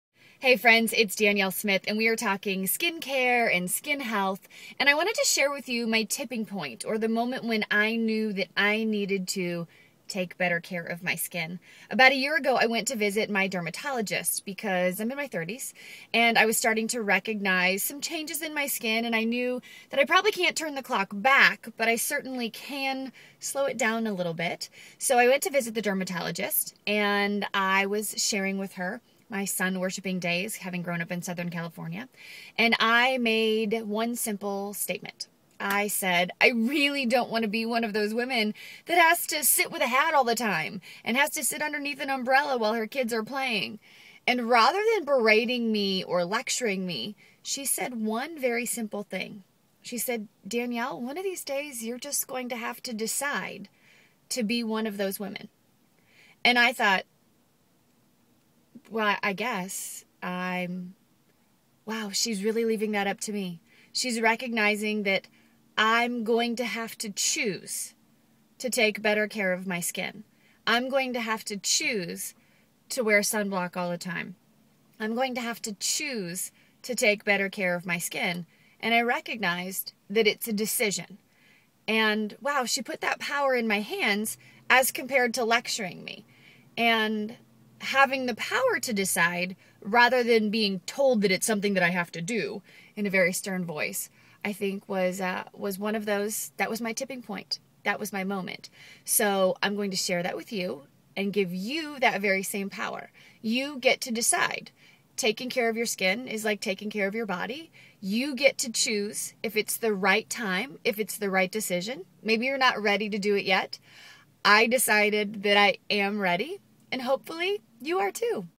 Skin Health Choice Therapon
Choosing to take care of my skin - what was my 'tipping point' or the moment that made me decide I needed to take better care of my skin?  It was a little something my dermatologist said to me...

Now, working as a 'Real Beauty Blogger' with Therapon Skin Health - I'm thrilled to have made this decision!